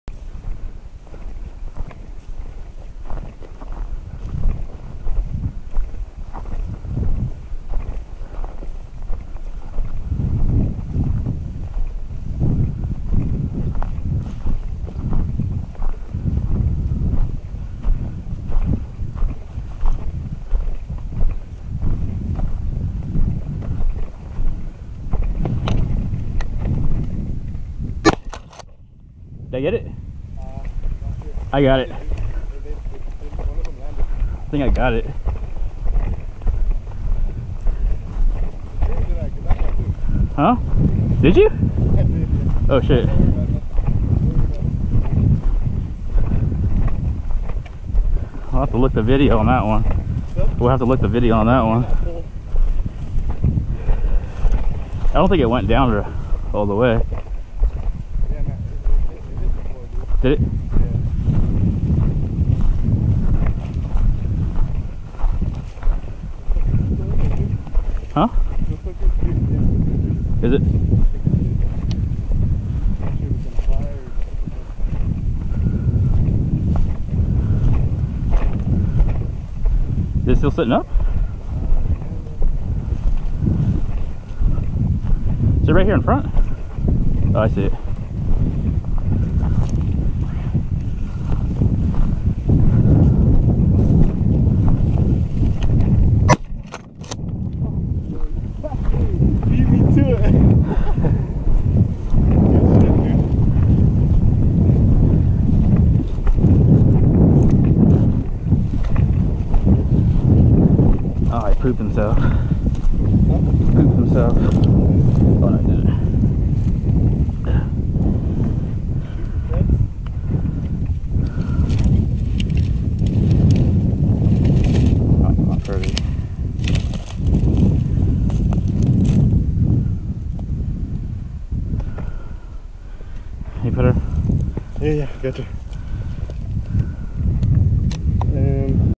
Walking Up (Jump Shooting Dove)
The dove action was a little slow, so me and my buddy got up and kicked some brush to get some doves in the air!